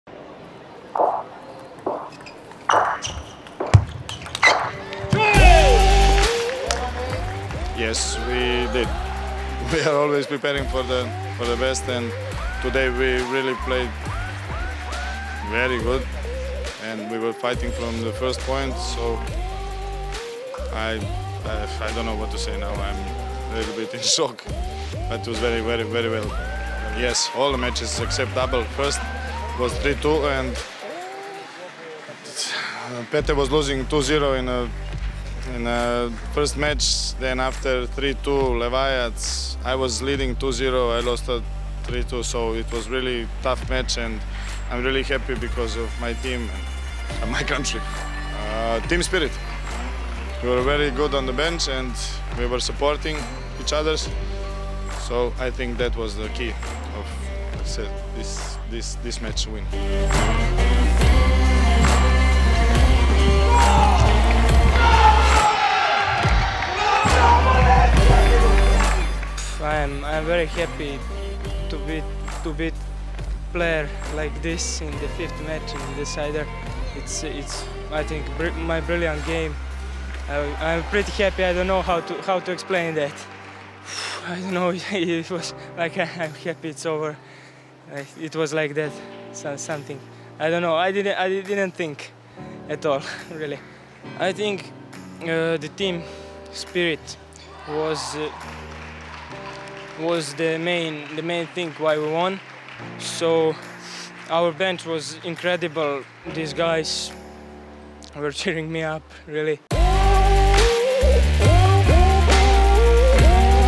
Interview Serbia | 2020 World Team Qualification
Listen to team Serbia 🇷🇸 after a sensational victory over Austria 🇦🇹3-2